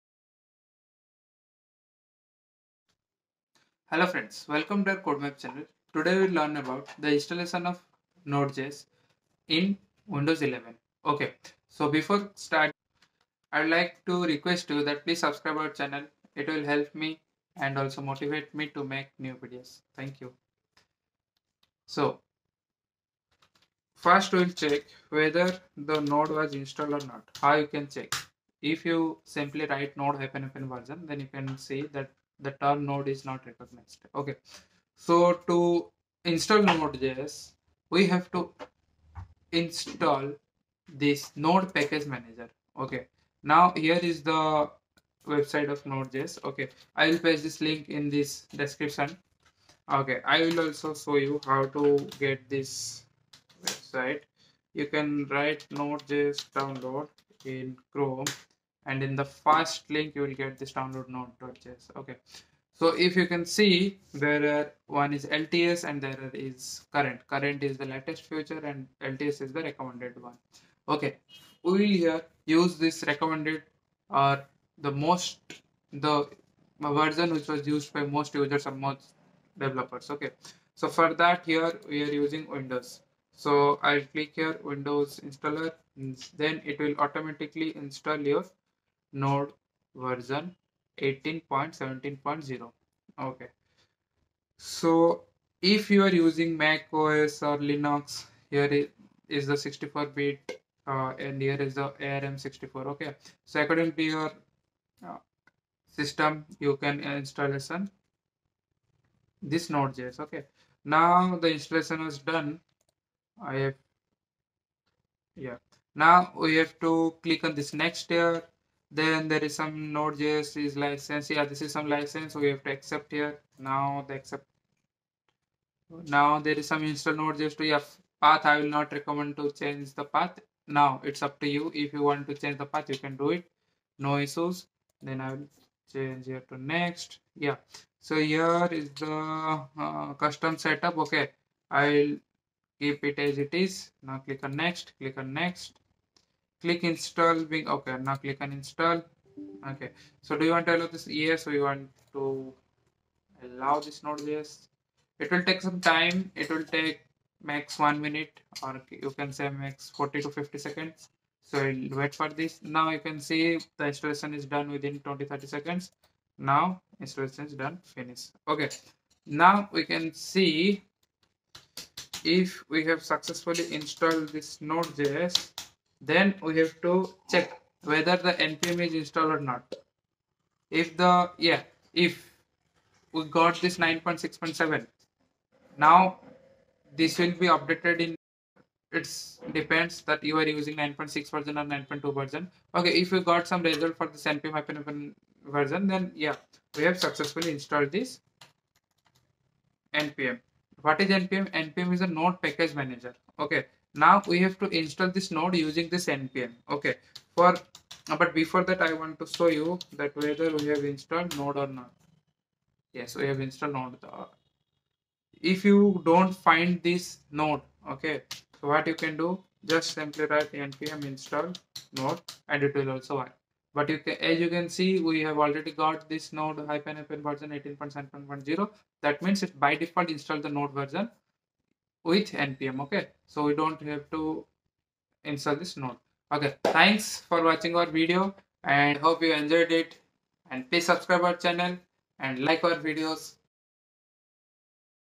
How to Install Node.js and NPM on Windows 11 [ 2023 Update] | NodeJS Installation
Learn How to install Node.js on Windows 11.  NodeJS is an open-source and cross-platform JavaScript runtime environment, developed by Ryan Dahl. To download Node.js, refer to the below link: